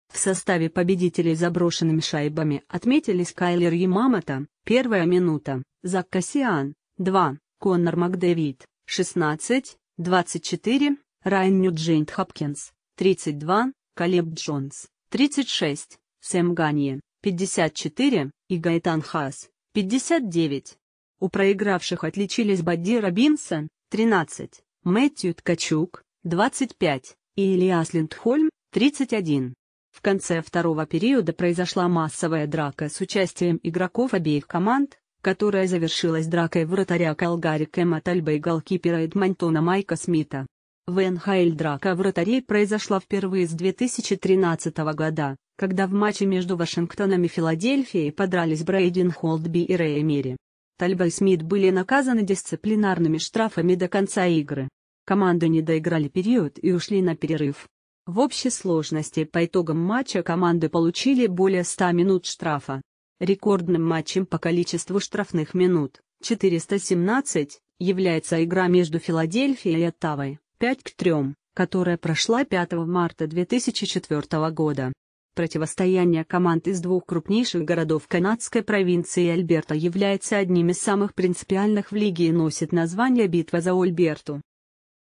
«Эдмонтон» и «Калгари» не доиграли период матча НХЛ из-за массовой драки
В составе победителей заброшенными шайбами отметились Кайлер Ямамото (1-я минута), Зак Кассиан (2), Коннор Макдэвид (16, 24), Райан Нюджент-Хопкинс (32), Калеб Джонс (36), Сэм Ганье (54) и Гаэтан Хас (59). У проигравших отличились Бадди Робинсон (13), Мэттью Ткачук (25) и Элиас Линдхольм (31).